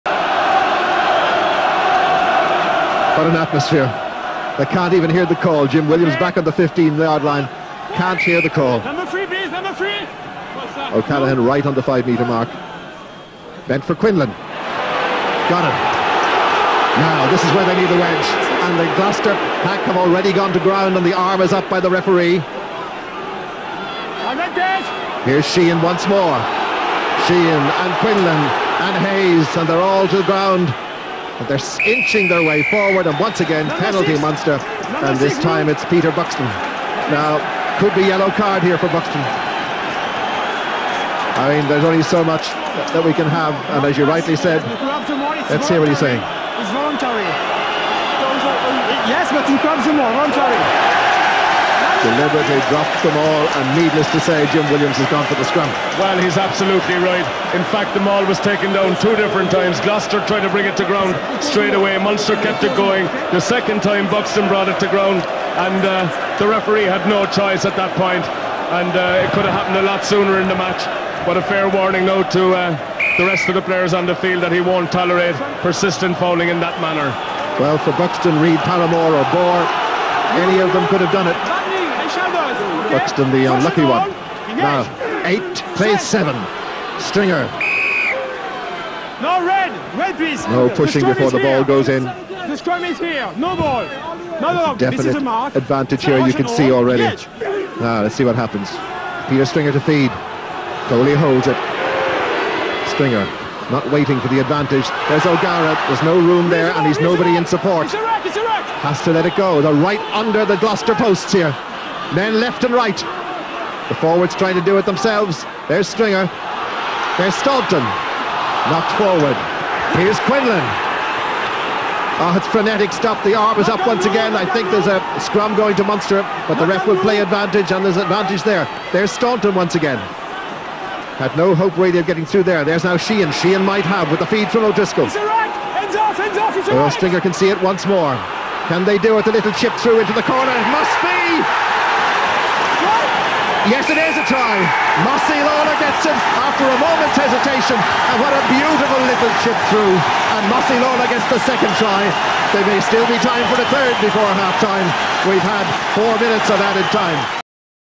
Mossie Lawlor, 2003
Mossie Lawlor scores against Gloucester

Munster v Gloucester, 20/01/03